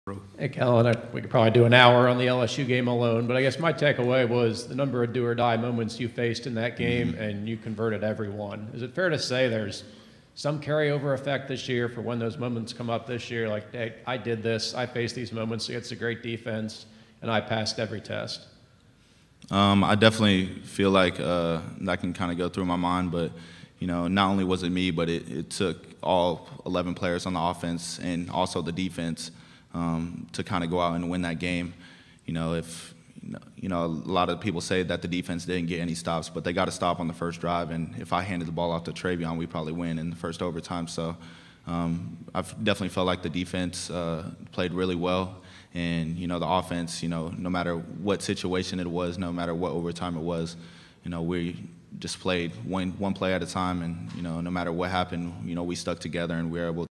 Kellen Mond talks about LSU 7 OT game
Texas A&M QB Kellen Mond briefly discusses the 2018 Texas A&M-LSU 7 overtime thriller which A&M won 74-72.